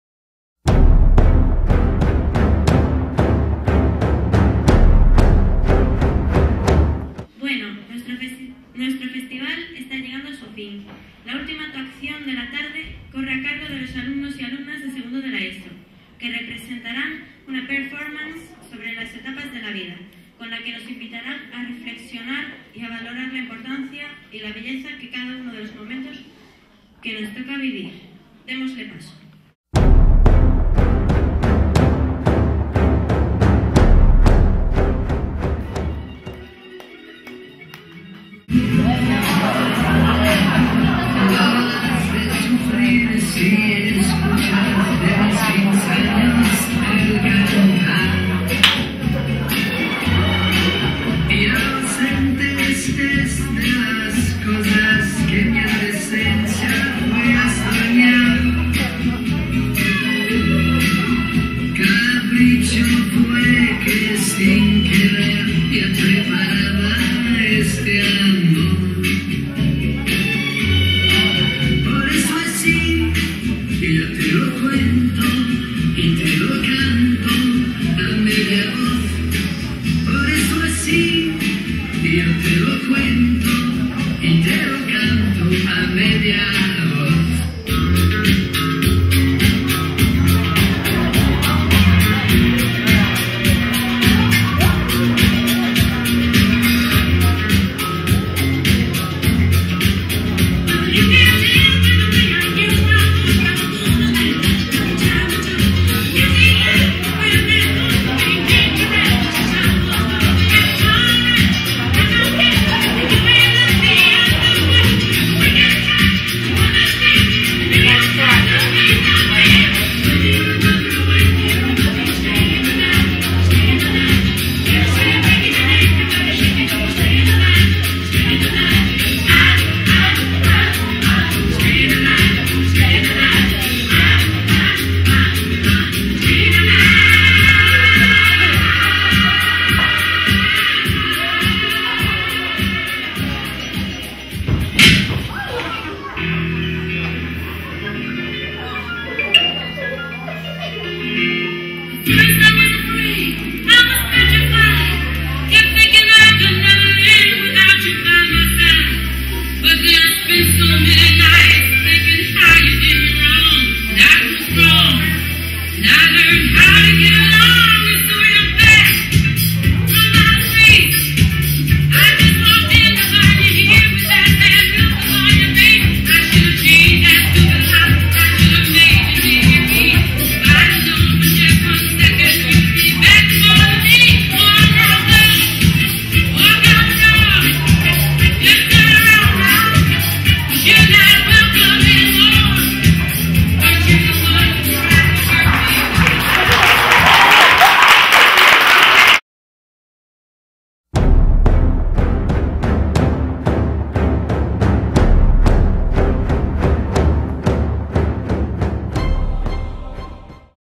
Colegio Doroteas Performance 2
Colegio Doroteas Performance "AS ETAPAS DA VIDA" estupenda representación de los Alumnos de 1º y 2º de la Eso